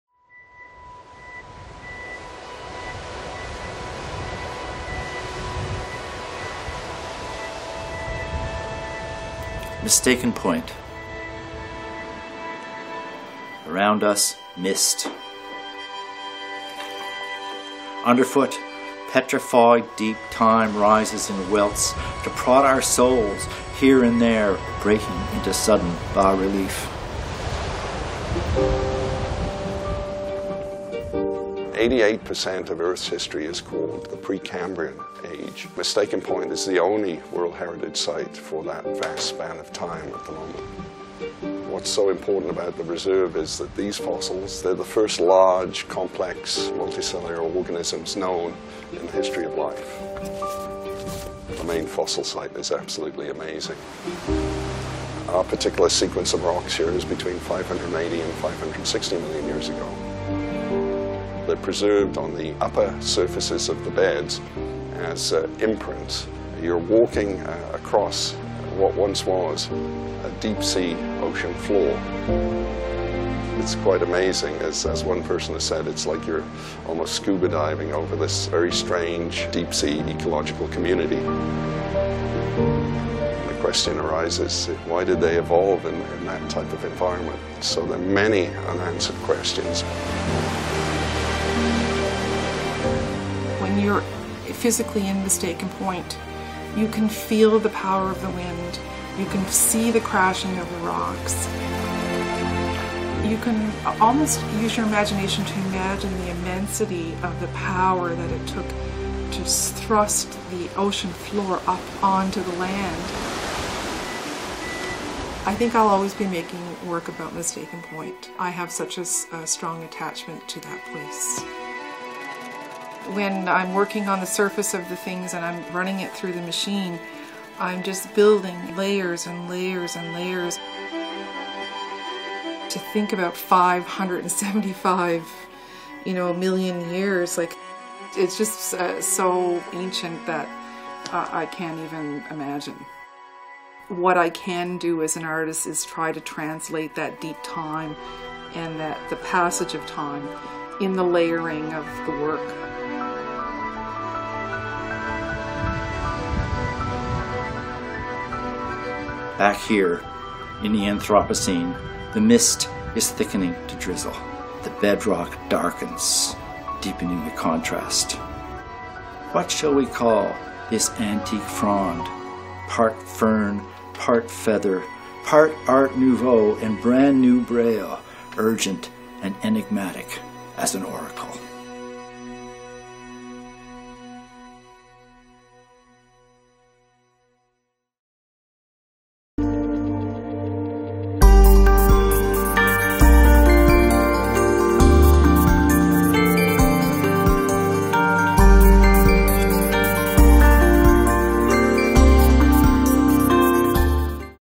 Discover Ancient Wonders on the Coast of Newfoundland and Labrador | National Geographic
In this romantic, eerie, and breathtaking landscape, you can find fossils of the first large, complex, multi-cellular organisms known in the history of life. Mistaken Point Ecological Reserve is a newly minted UNESCO World Heritage Site in Newfoundland and Labrador and is home to these 580 million year old fossils.

Although the rock ledges where these ancient fossils are embedded sit high above the crashing coastal waves, they once were situated at the bottom of the ocean. Mistaken Point, and all it’s wonders, serve as a muse for some of the region’s talented artists.